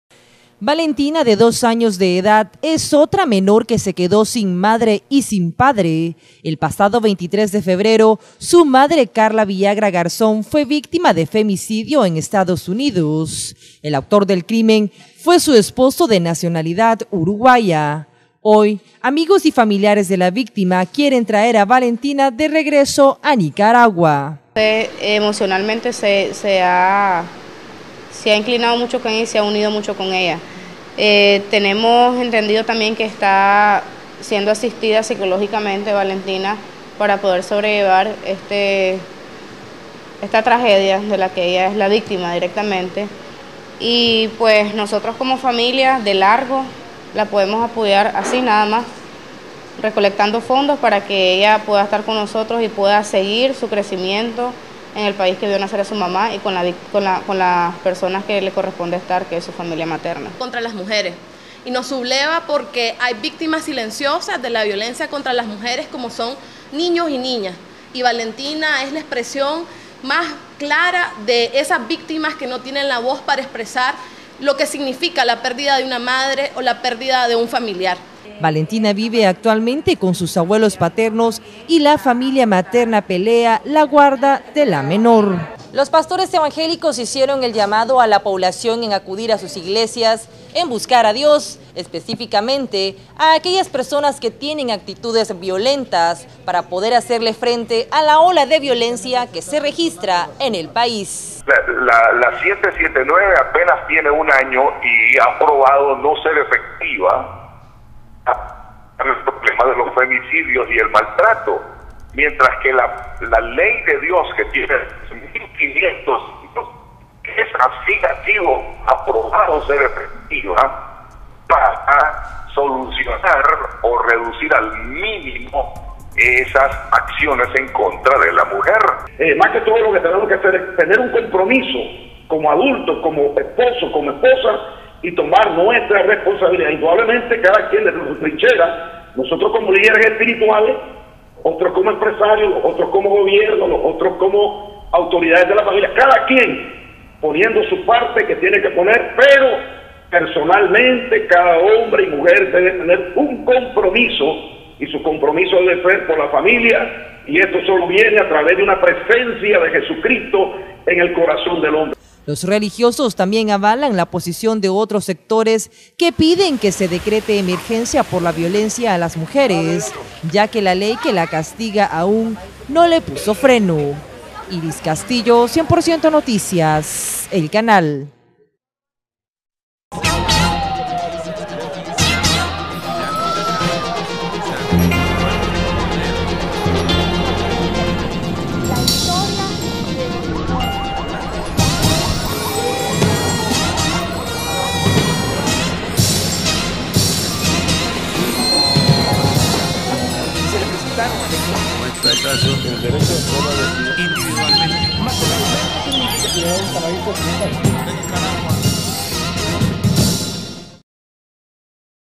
Femicidio Nicaraguense en EU, Karla José Villagra Garzón
Karla José Villagra Garzón, de 32 años de edad, fue asesinada por su esposo, el uruguayo Abayuba Rivas, de 39 años, en el condado de Elizabeth, en Nueva Jersey, Estados Unidos.

El crimen sucedió hace tres semanas cuando Rivas discutió con Karla y luego denunció ante las autoridades que ella había desaparecido tras salir a comprar a una farmacia.

Sin embargo, el cuerpo de Karla Villagra fue hallado por la policía esta semana en una vivienda abandonada en Southern Boulevard, en Chatham Township, condado de Morris, a 17 kilómetros de su casa.

Una autopsia realizada por la unidad forense del condado reveló que Karla José Villagra murió por estrangulamiento, y que además fue golpeada en la cabeza con un objeto contundente.